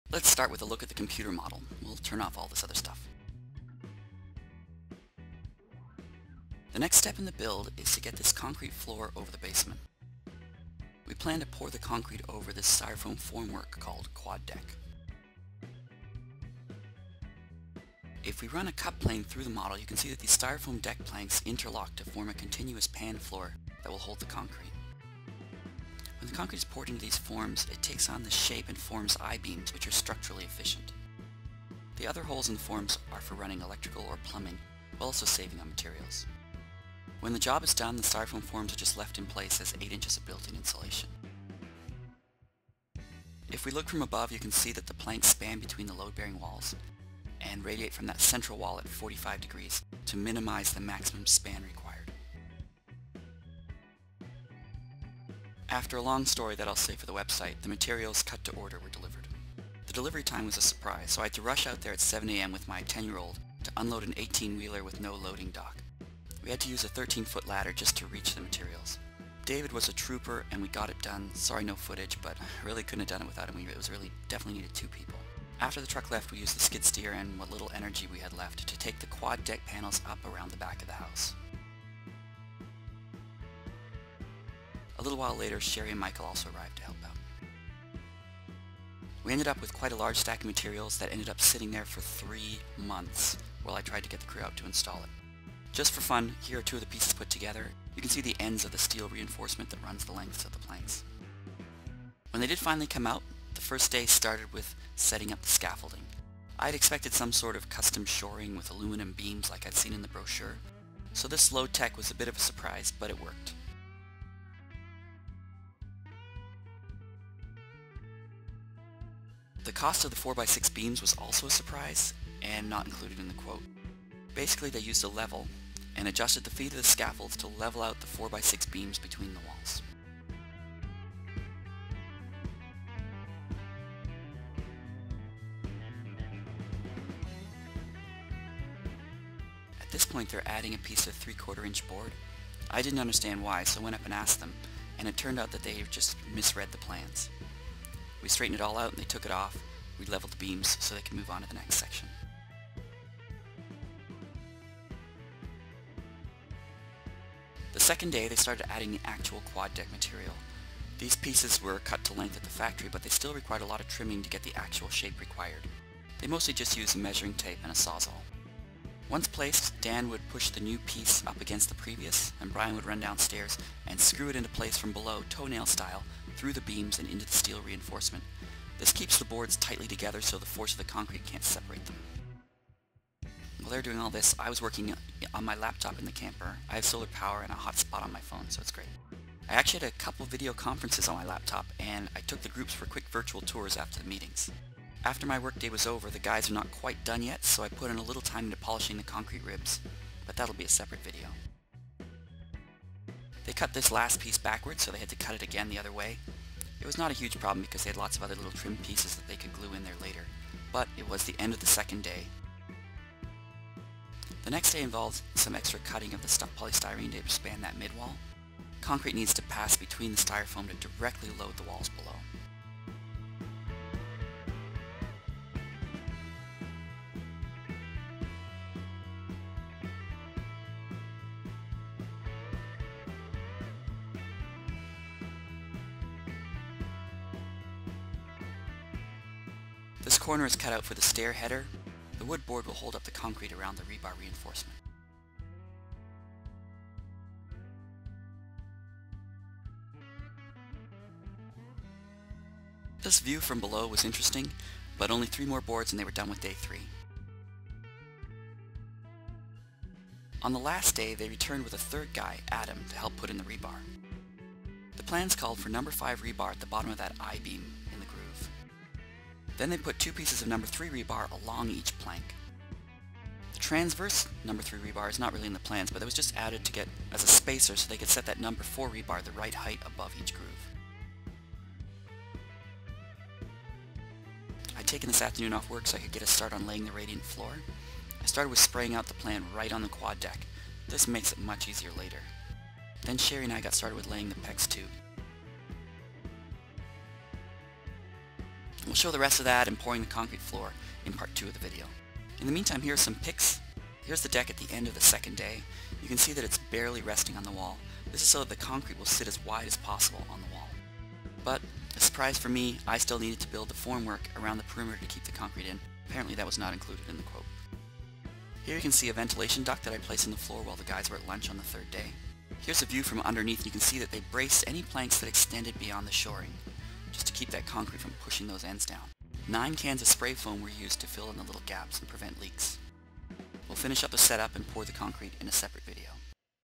20.0 Quad Deck Install (part 1 of 2)
Getting the quad deck installed as a floor in my earth sheltered home, for more details, check out